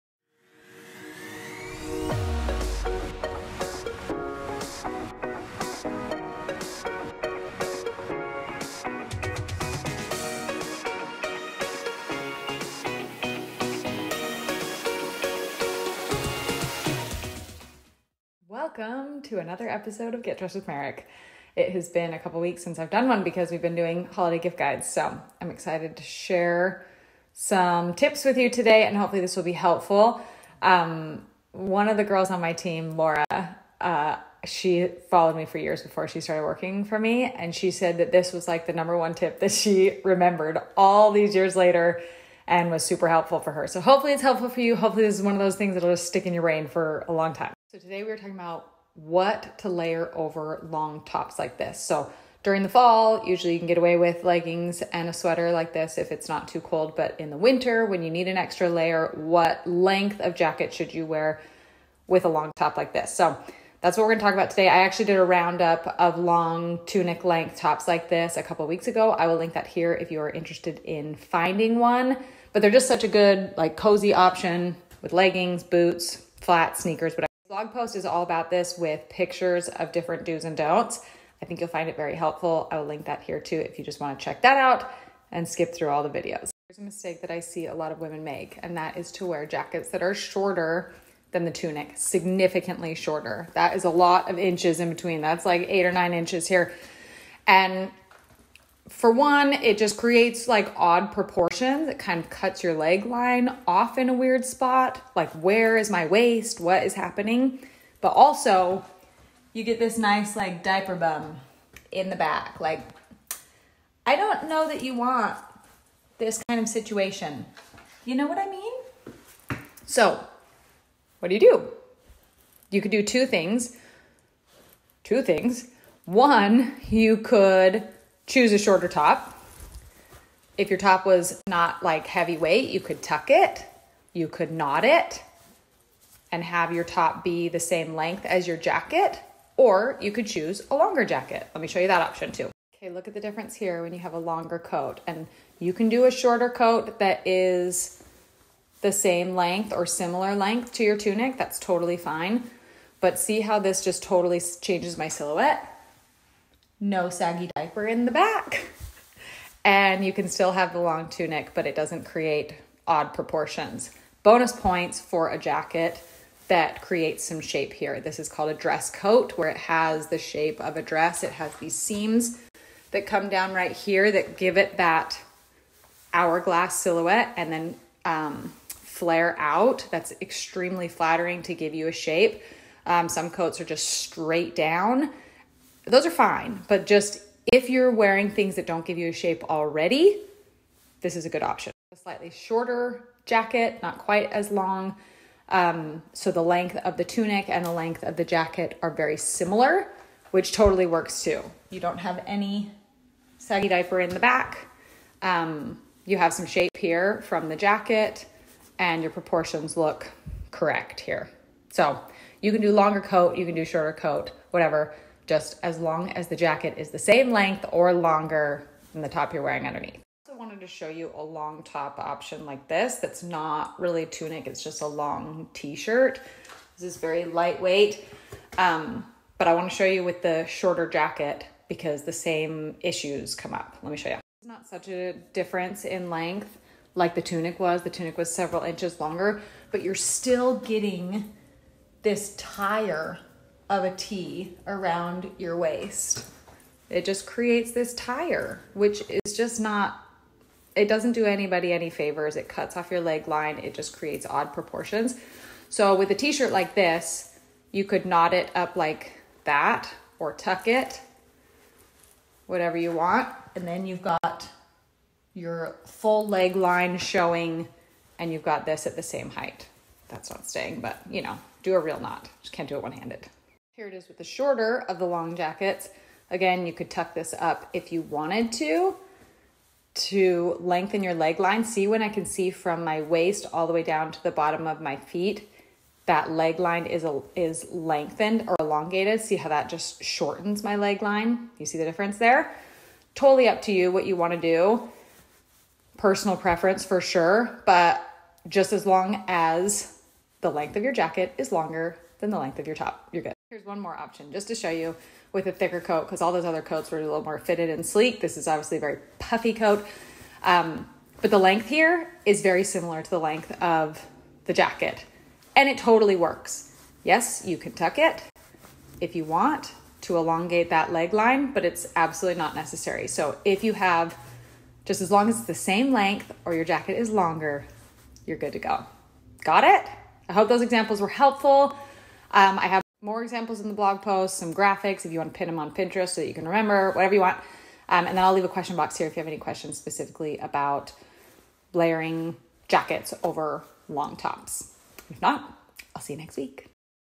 What Jackets to Wear with Different Tops
If you’re wearing a long sweater, tee, or tunic in the winter, you might need a jacket or coat with it. Here are some easy tips to help you layer these longer tops well.